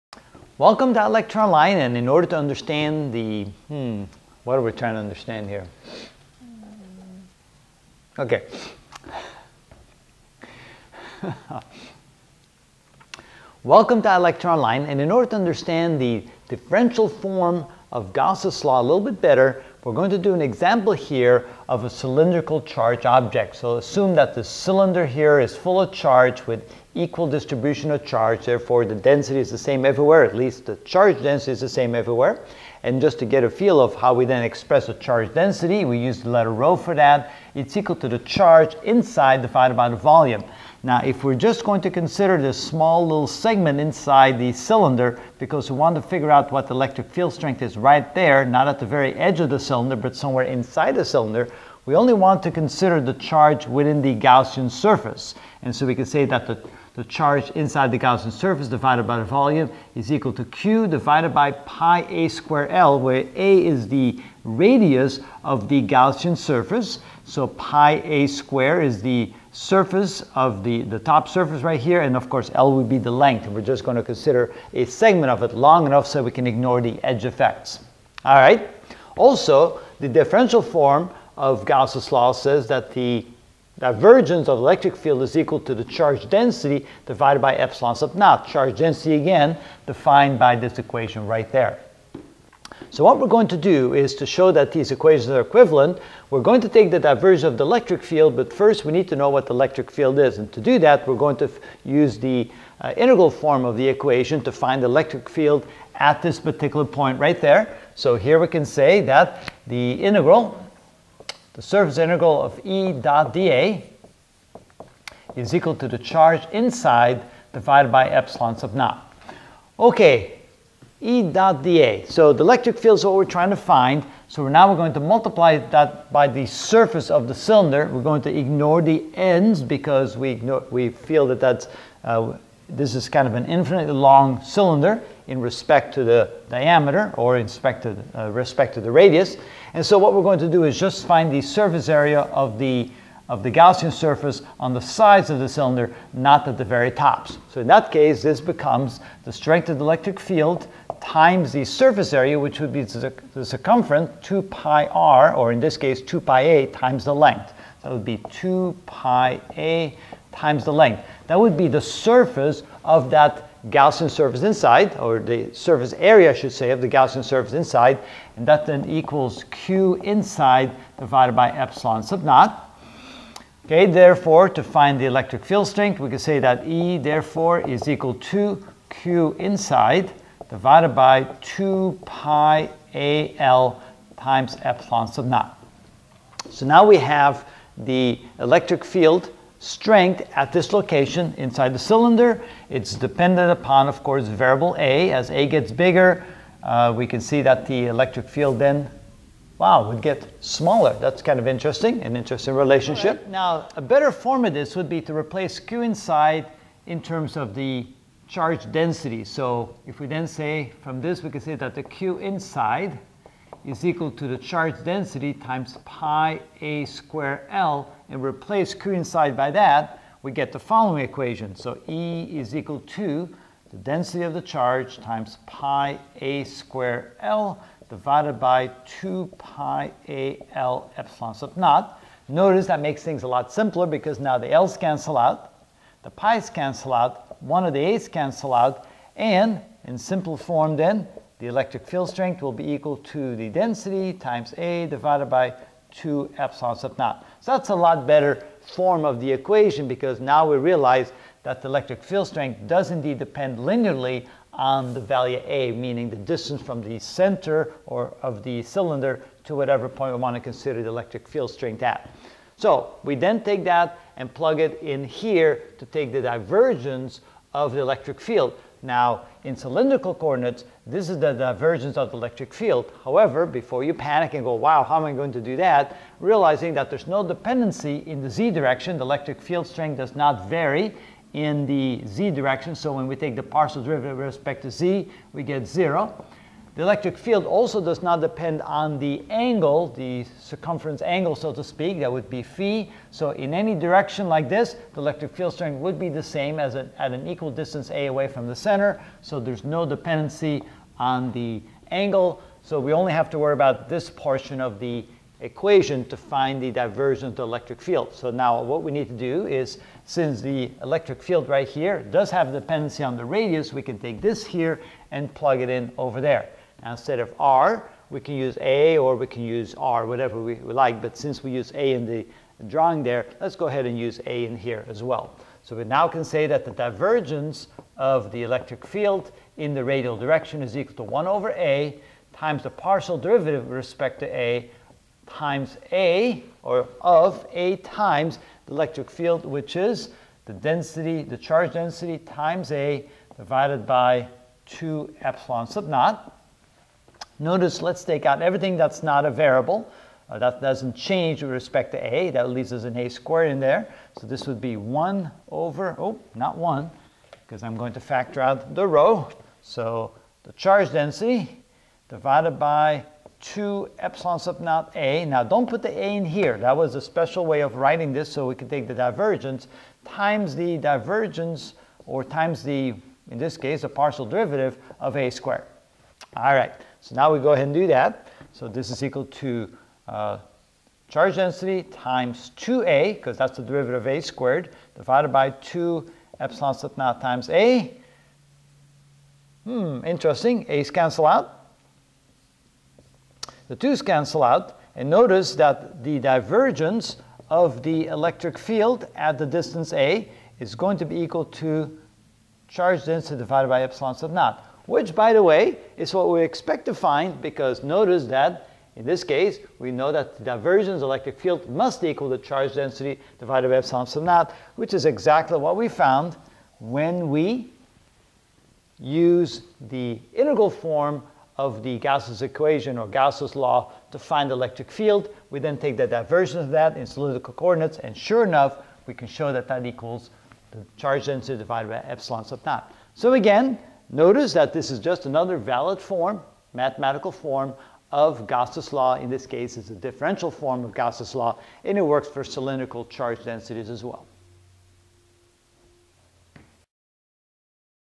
Physics 46 Maxwell's Equations (15 of 30) Differential Form of Gauss' Law: 7
In this video I will explain the cylindrical charge of the differential form of Gauss Law.